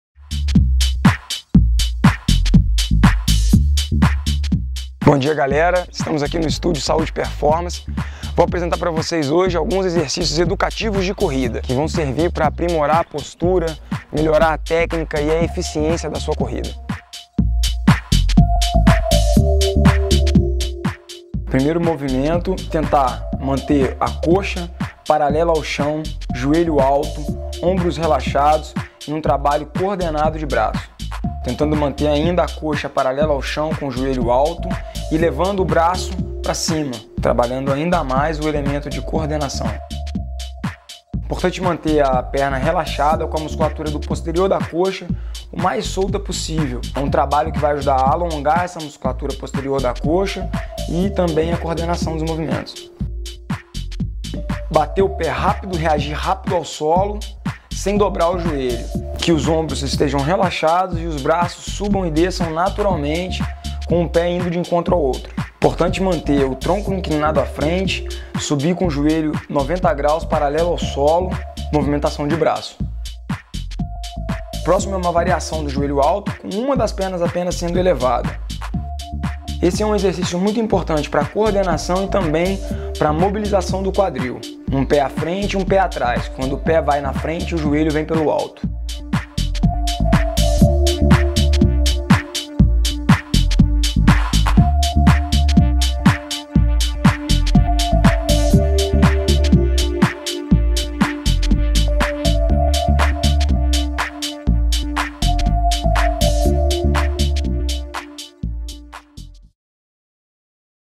Exercicios educativos de corrida - Saúde Performance
Neste vídeo apresentamos uma sequencia com alguns movimentos educativos para corrida. A intenção destes exercícios é melhorar a postura, o rendimento e a técnica de sua corrida.